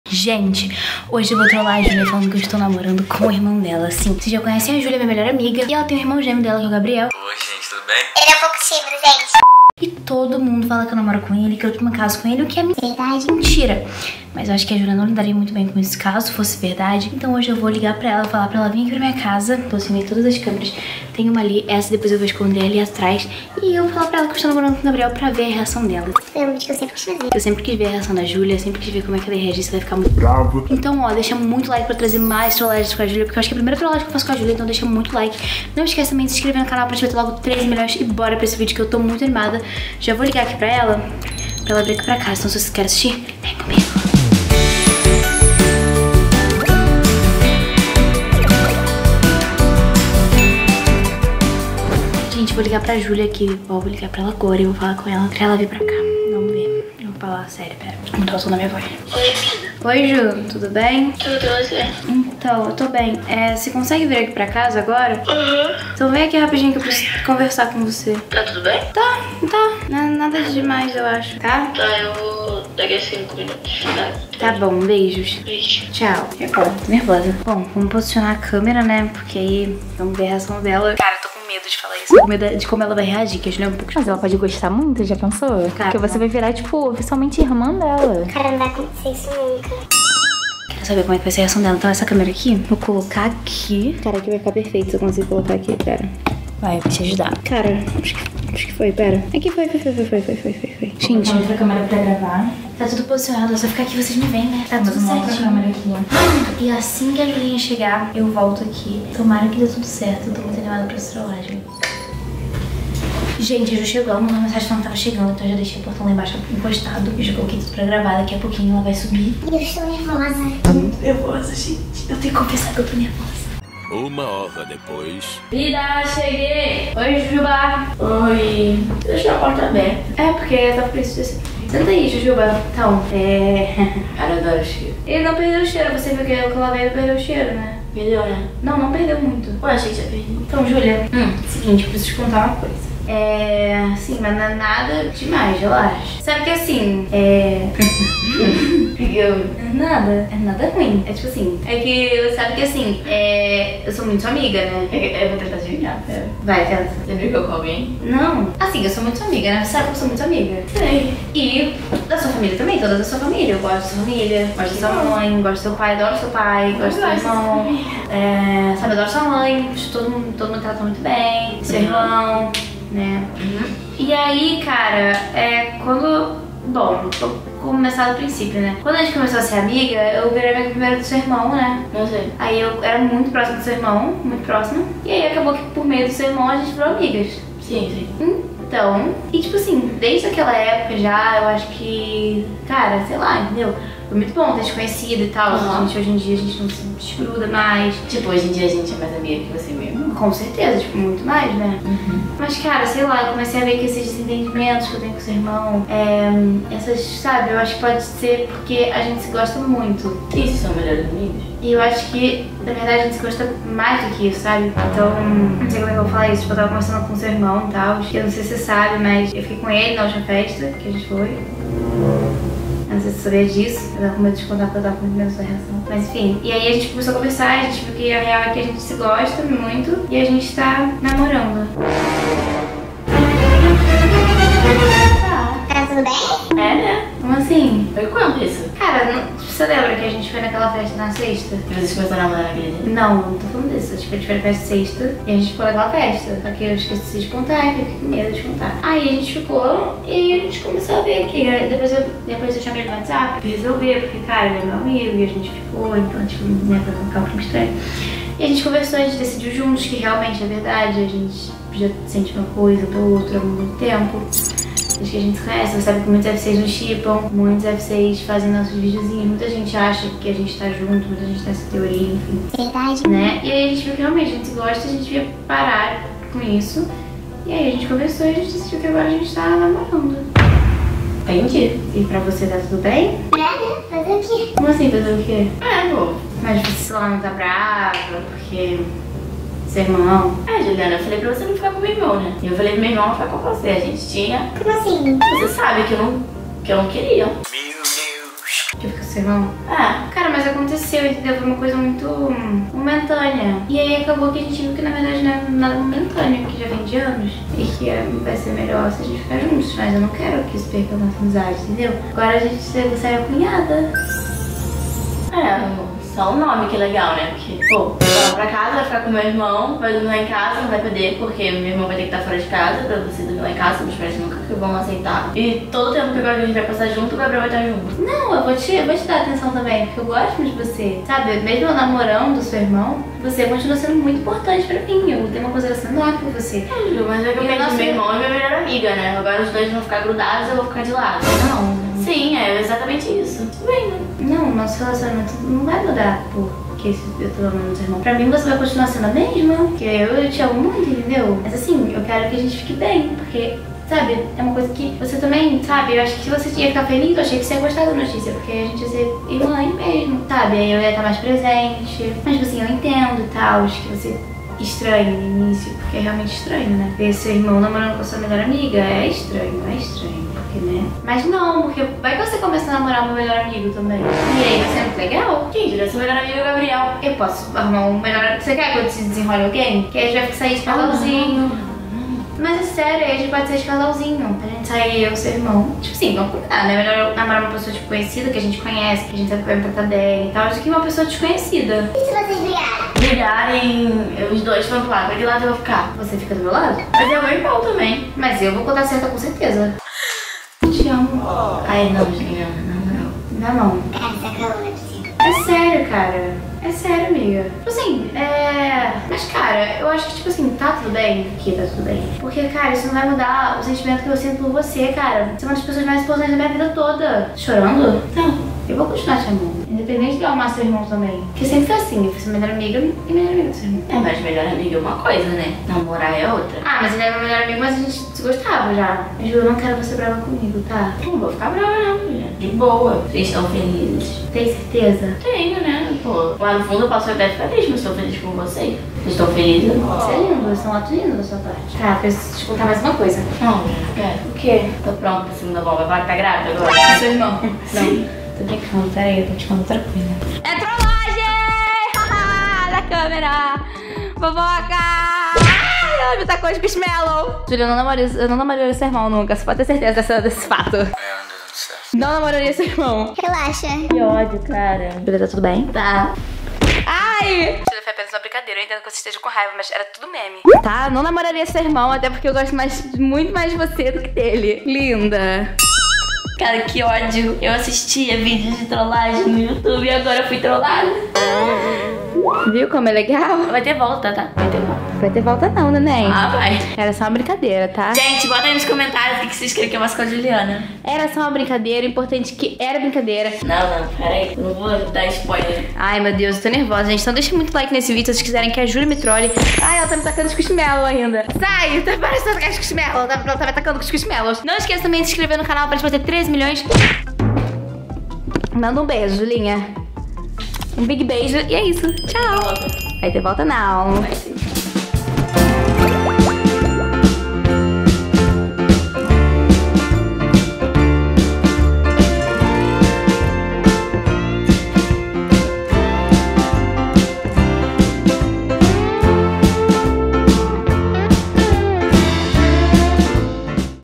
ESTOU NAMORANDO O IRMÃO DA MINHA MELHOR AMIGA, CONTEI TUDO PRA ELA! - JULIANA BALTAR
Galera, no vídeo de hoje eu decidi trollar minha amiga Júlia fingindo que estou namorando com o irmão dela! Qual será a reação dela?